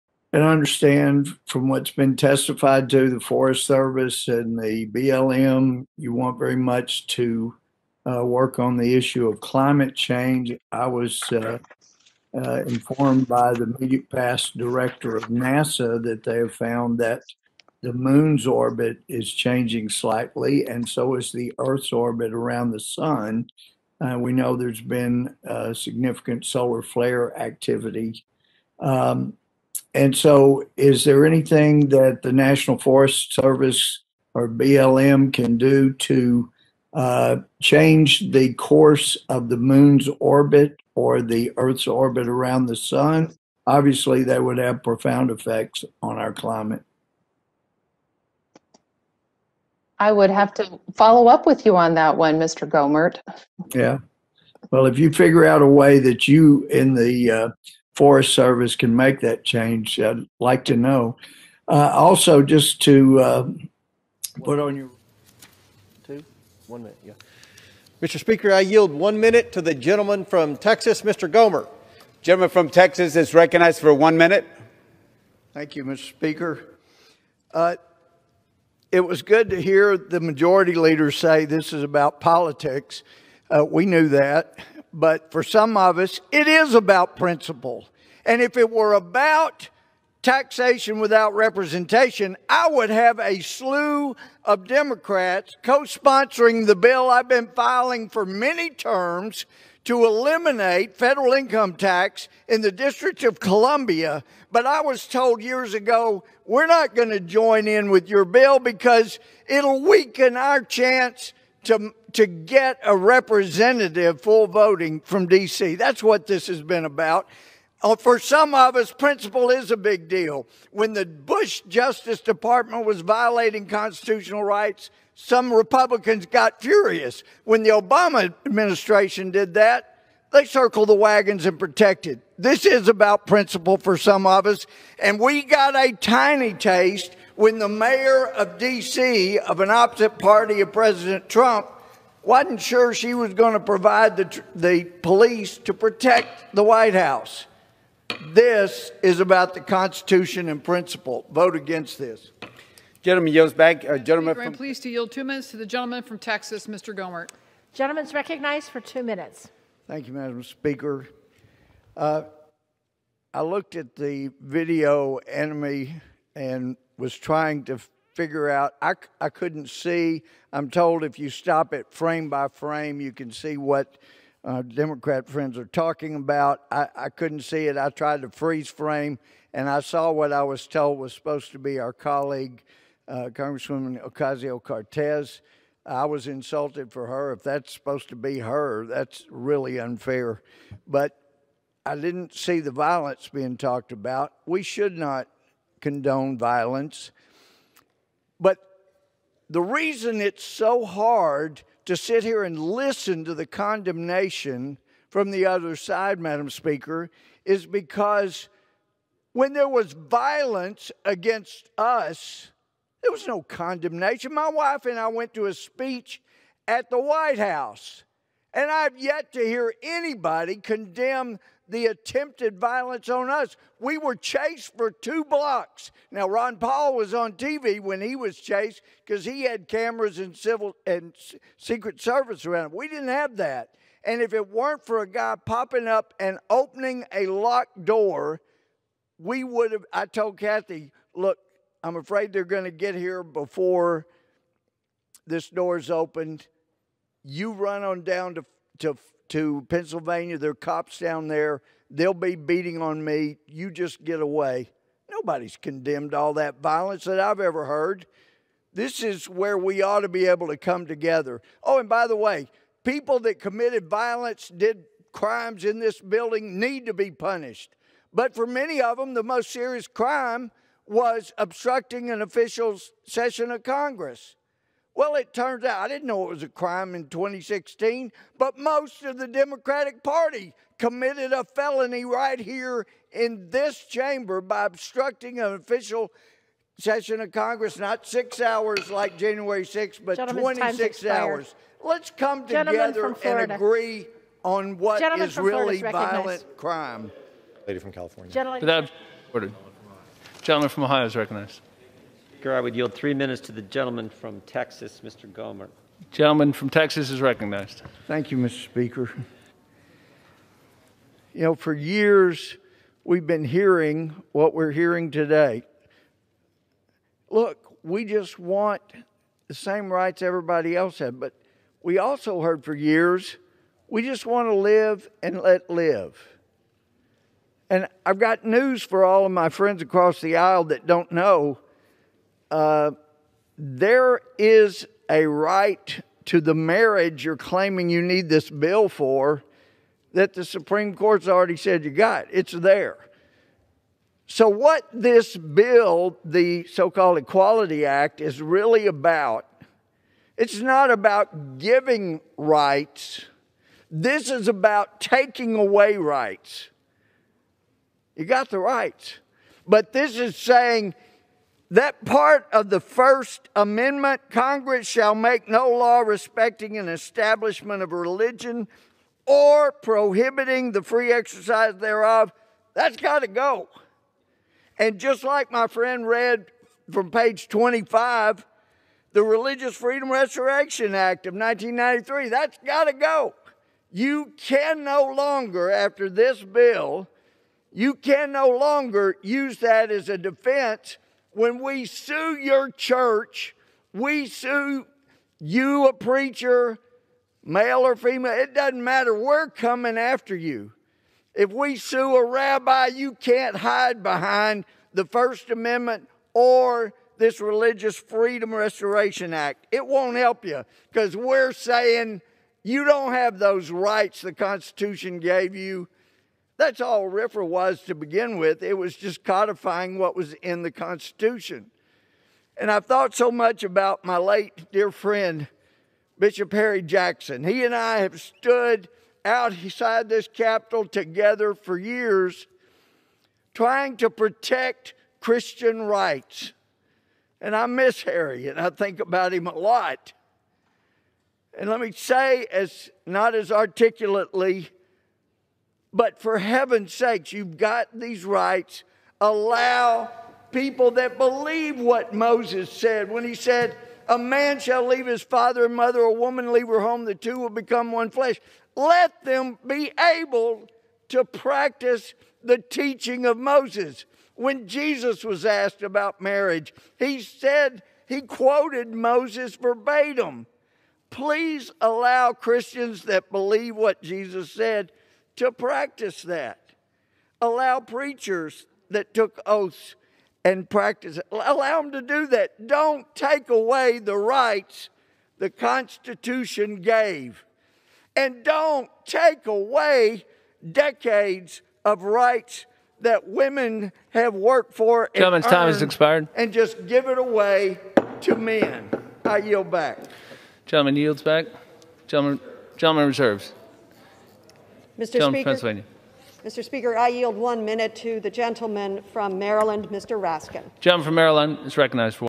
Louie Gohmert Inquires About Orbits, Makes Passionate Floor Speeches In Memorable Year | 2021 Rewind
As 2021 comes to an end, watch some of the top moments from Rep. Louie Gohmert (R-TX).